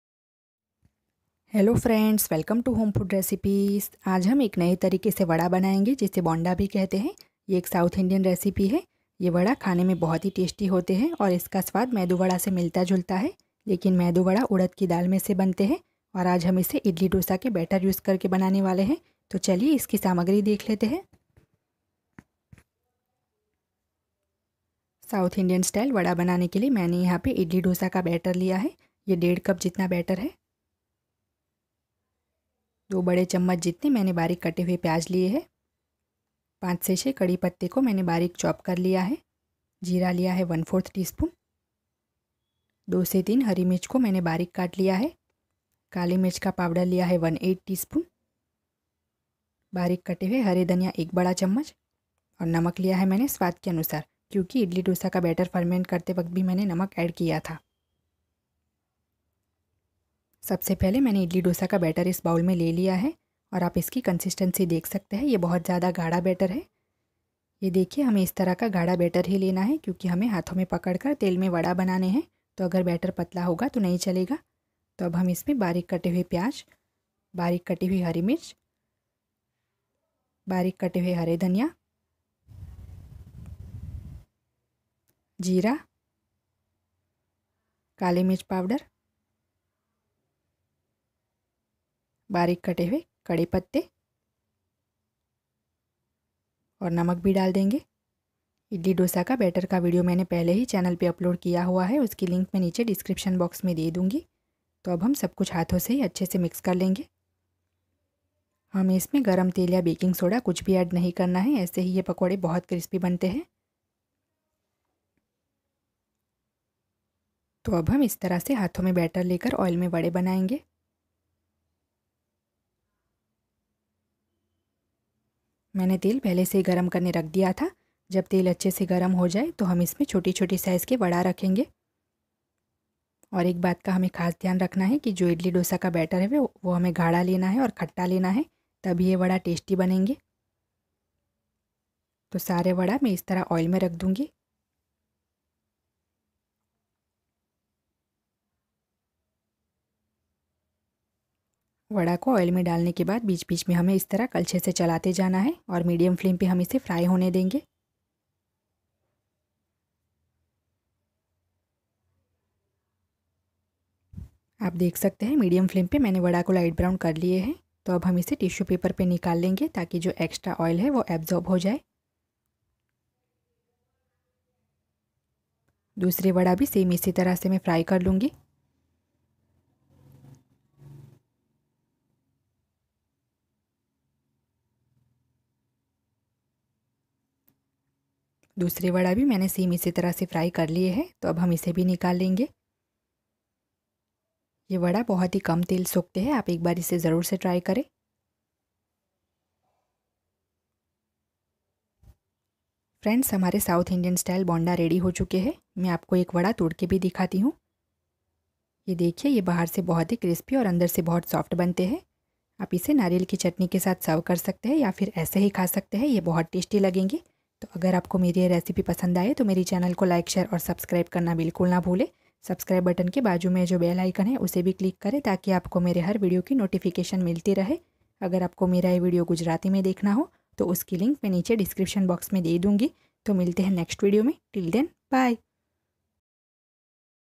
डोसा के बेटर मेसे सिर्फ दस मिनट मे बनाये वडा/vada banane ka tarika /bonda/south Indian bonda recipe
description

Home food recipes

Vada/bonda recipe ingredients :-

Idli dosa batter 1 & 1/2 cup
Chopped onion 2 tbsp
Chopped curry leaves 5 to 6
Cumin seeds 1/4 tsp
Chopped green chillies 2 to 3

હવે ઘરેજ બનાવો બજાર કરતા ઓછા ખર્ચ મા મુંબઈ ફેમસ મટકા ઢોંસા

જો ઈડલી ઢોંસા નુ ખીરુ આમ બનાવશો તો ૧૦૦% ઈડલી પોચી અને ઢોંસા ક્રિસ્પી જ બનશે